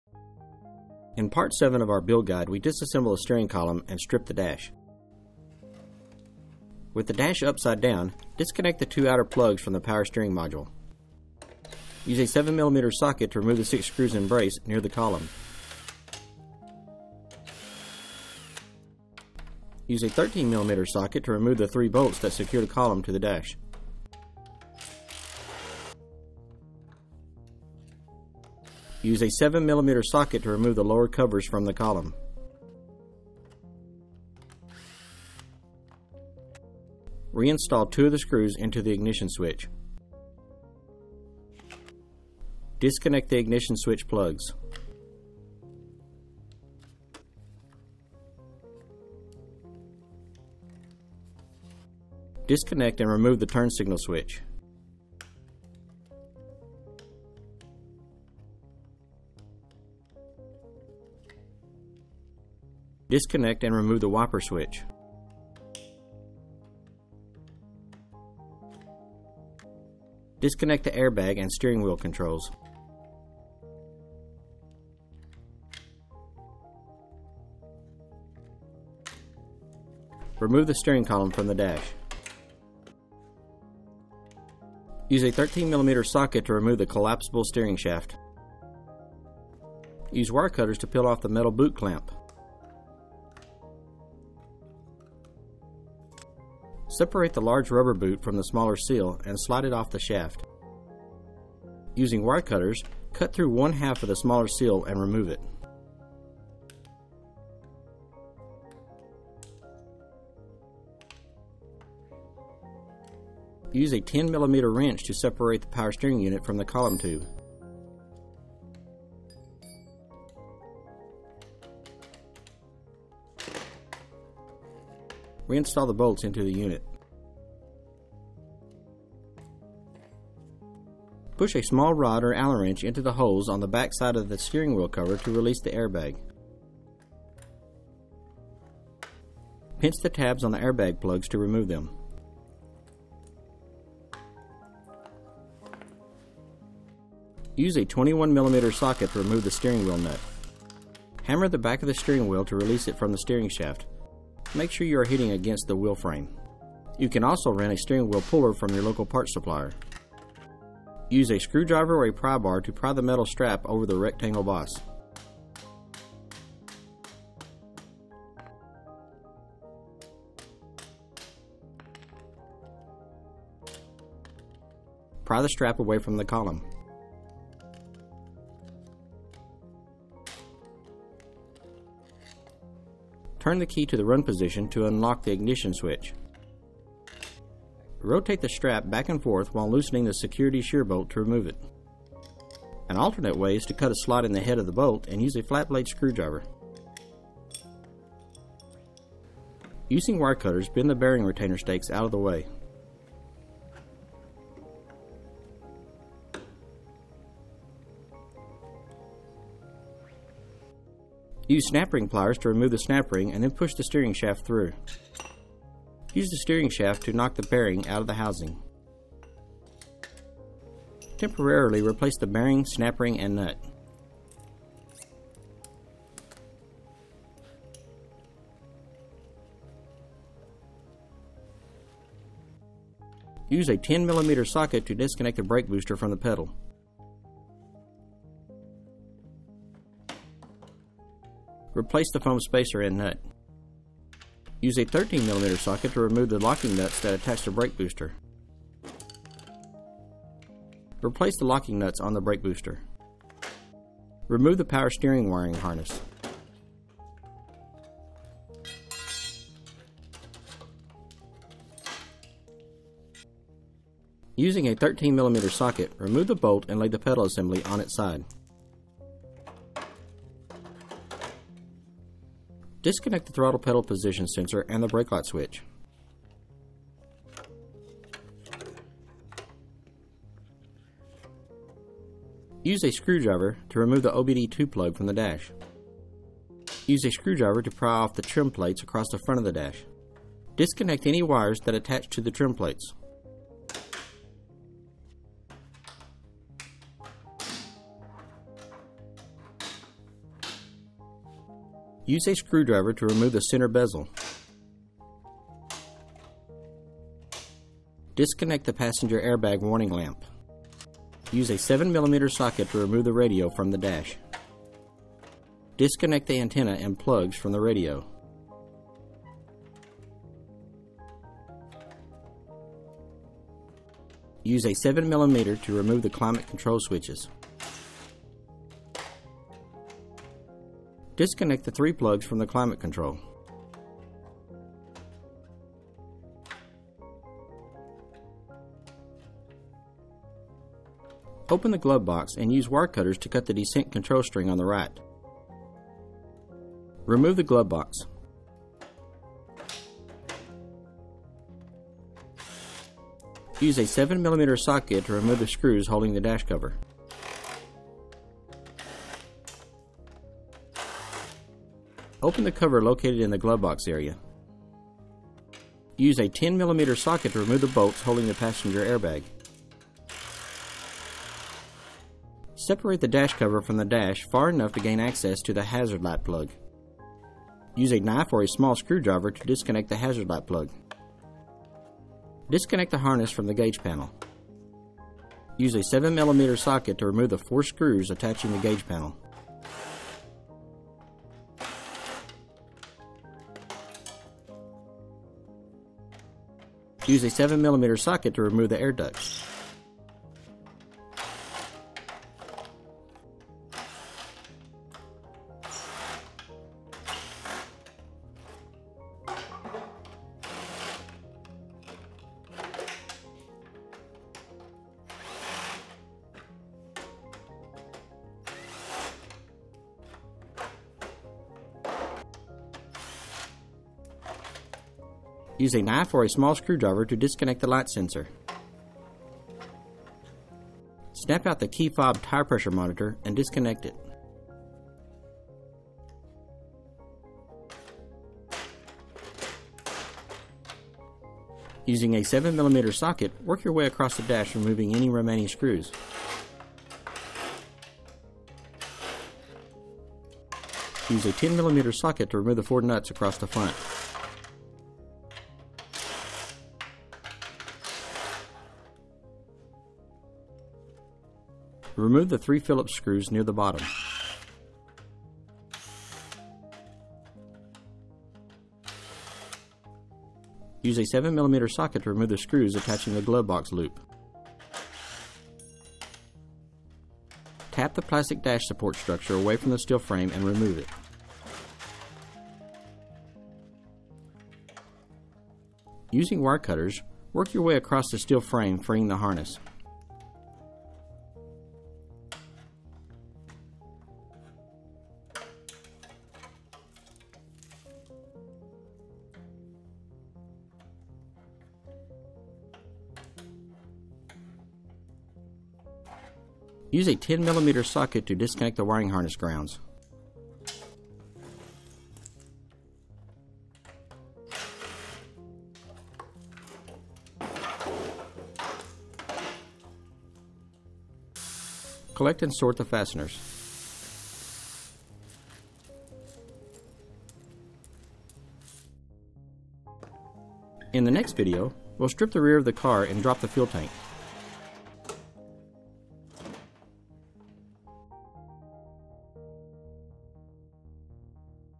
DF Goblin Build Guide Part 7 - Disassembling the Dash
Learn how to build your own Goblin kit car by following our DF Goblin Build Guide series.

In this part, we disassemble the steering column and strip the dash.

Check back for Part 8 where we strip the rear of the donor and drop the tank.

- for more information about the DF Goblin kit car

- check out DF Goblin build logs and other discussions